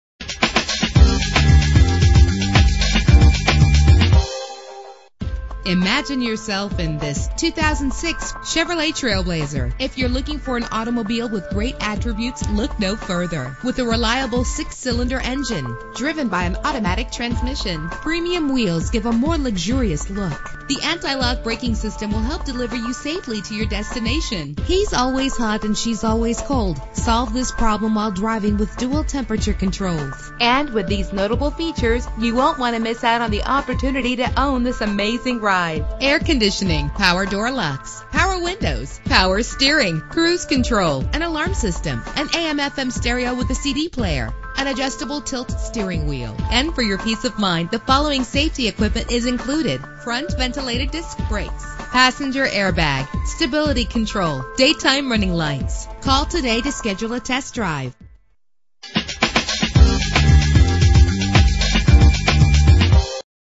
2006 Chevrolet TrailBlazer - WINTER HAVEN FL
Year: 2006
Make: Chevrolet
Model: TrailBlazer
Trim: LS
Engine: 4.2 liter inline 6 cylinder 24 valve
Transmission: 4-Speed Automatic
Color: Superior Blue Metallic
Mileage: 76028
Address:
299 CYPRESS GARDENS BLVD